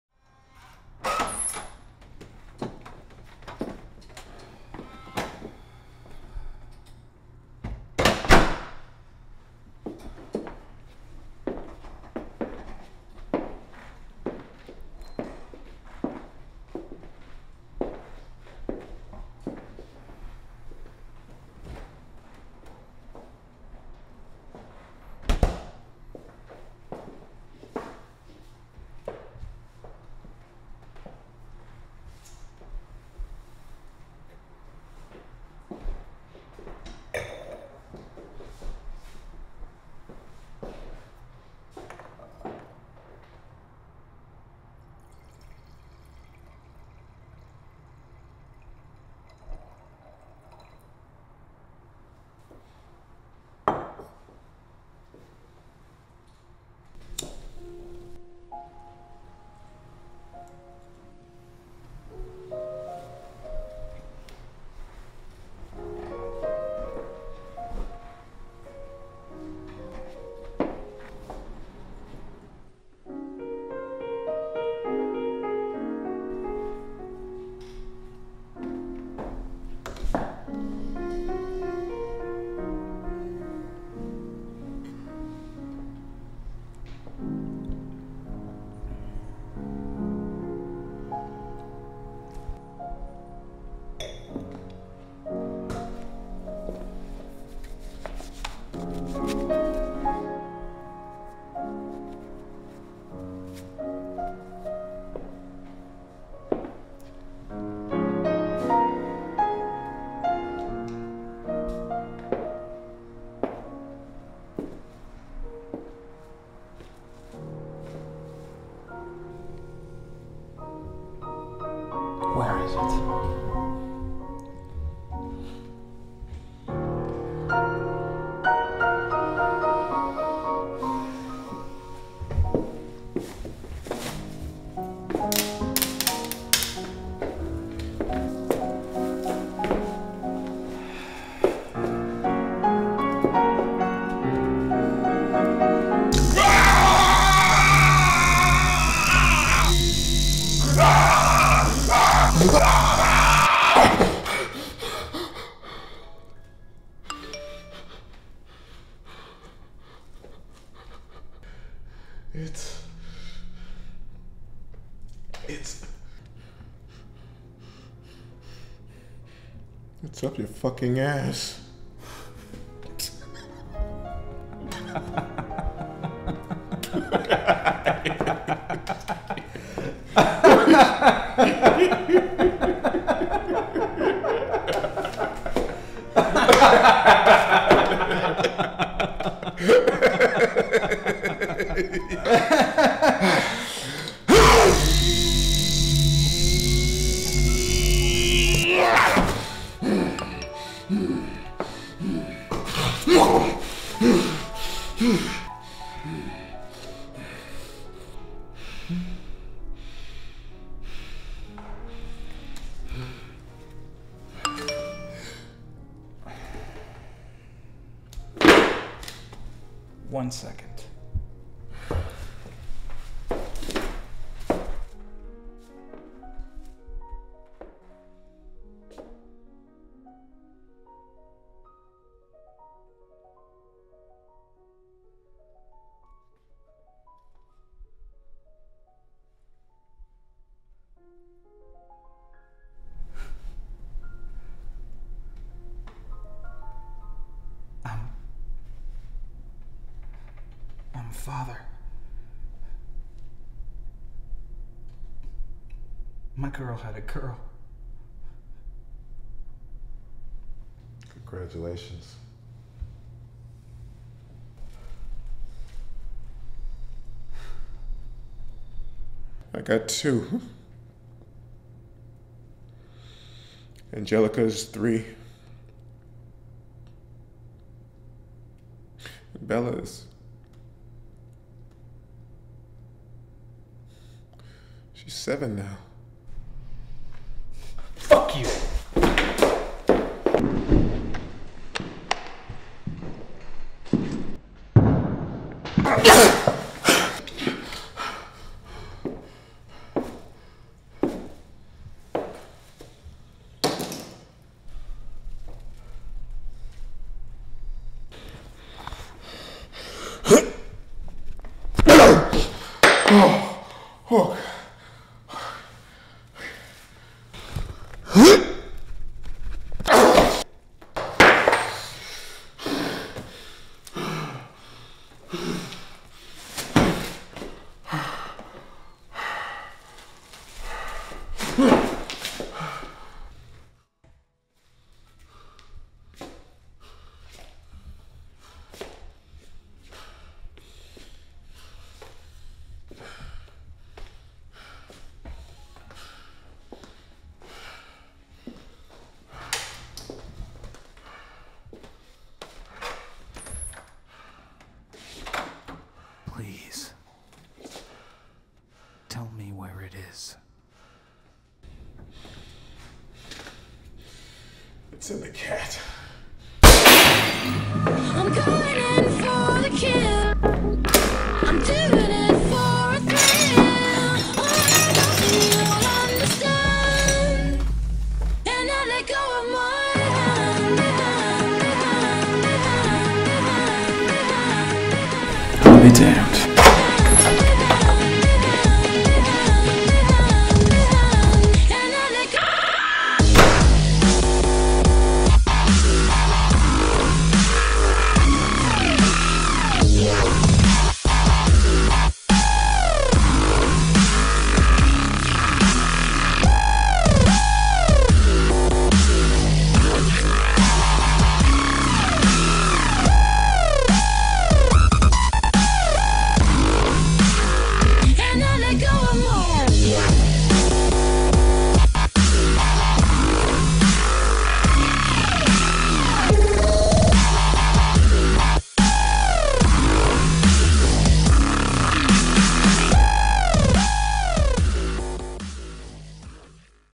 The Hit (shortened)
Here is "The Hit" after working with the extraordinary talents of my professor, Val Kuklowsky from LA Film School.  This version features less dialogue and more visual storytelling.  Which one is better?  Best, Geoff Erwin